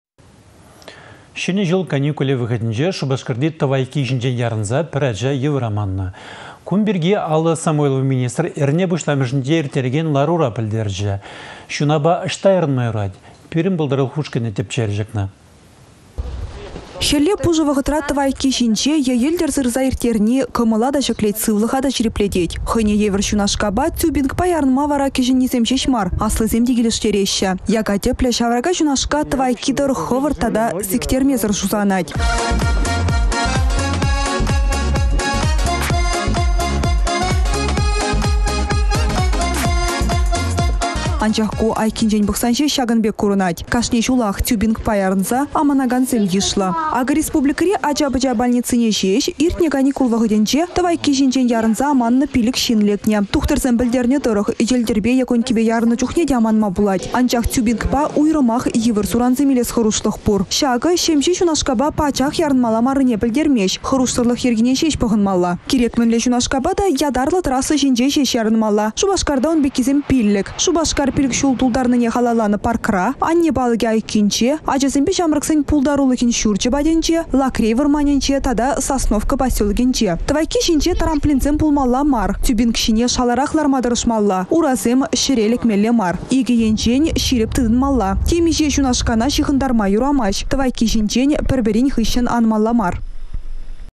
Çунапа ăçта ярăнма юрать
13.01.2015
Çĕнĕ çул каникулĕ вăхăтĕнче Шупашкарти тăвайкки çинчен ярăнса пĕр ача йывăр аманнă. Кун пирки Алла Самойлова министр эрне пуçламăшĕнче ирттерекен ларура пĕлтерчĕ. Çунапа ăçта ярăнма юрать — пирĕн пултарулăх ушкăнĕ тĕпчерĕ.